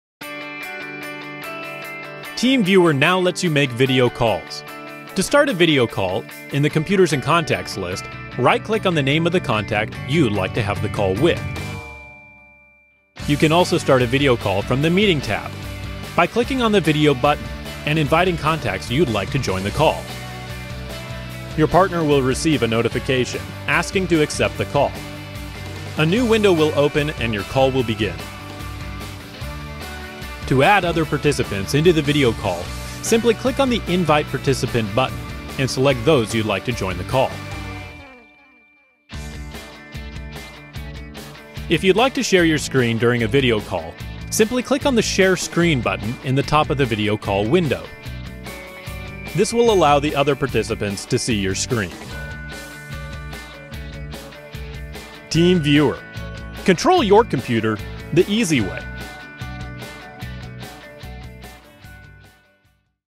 TeamViewer 10 - How to Make a Video Call
TeamViewer 10 now lets you make video calls. You can talk with multiple partners at once and even start screen sharing from within the video call window.